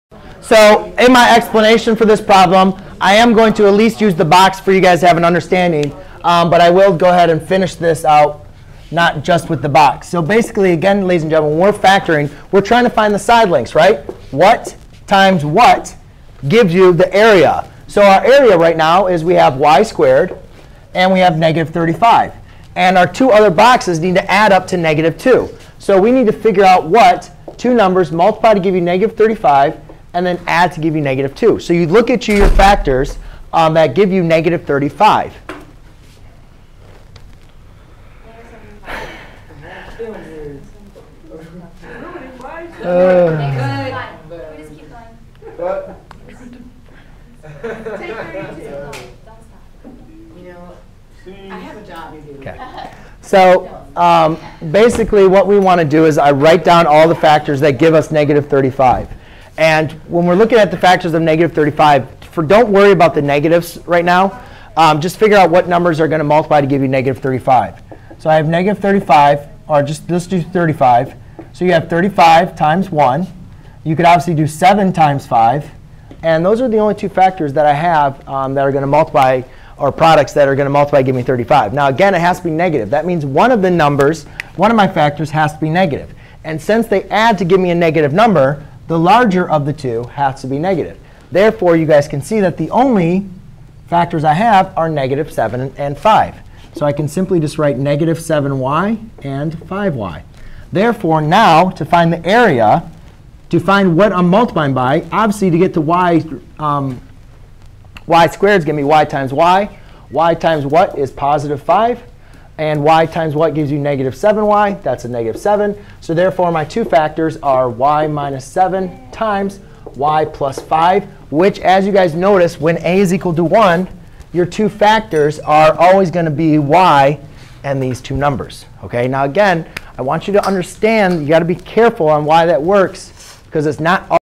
Learn how to factor a trinomial using a rectangle
👉Learn how to factor quadratics. A quadratic is an algebraic expression having two as the highest power of its variable(s). To factor an algebraic expression means to break it up into expressions that can be multiplied together to get the original expression.

To factor a quadratic with three terms and the coefficient of the squared variable is 1, all we need to do is to find two numbers which when multilied together gives the constant term (the term with no variable) and when the two numbers are added gives the coefficient of the the term whose variable has no power.

After getting the two numbers, we then write the variable with the two number in separate parenthesis.